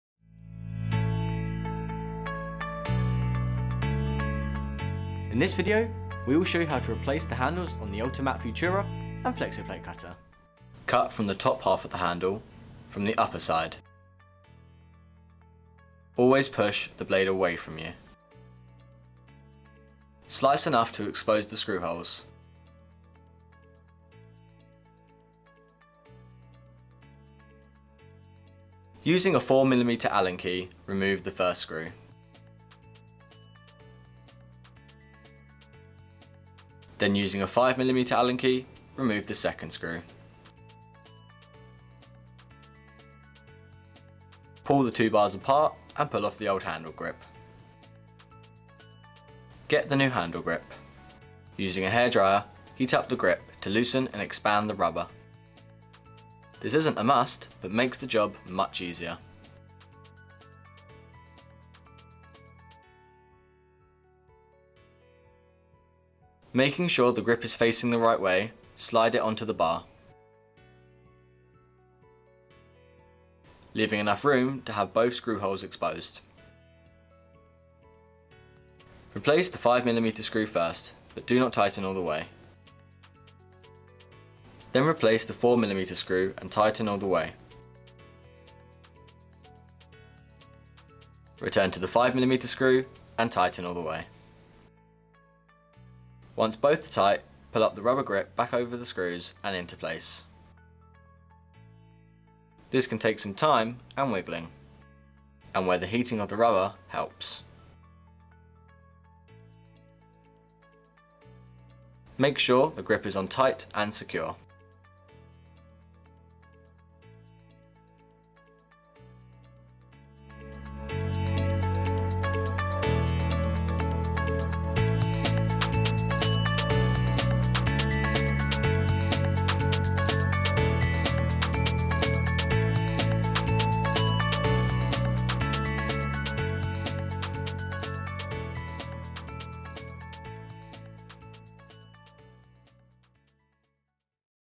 Ultimat Futura and Flexo Plate Cutter | How to replace the handle grips
This video shows you how to correctly remove and replace the handle grip on your Keencut Ultimat Futura and Flexo Plate Cutter.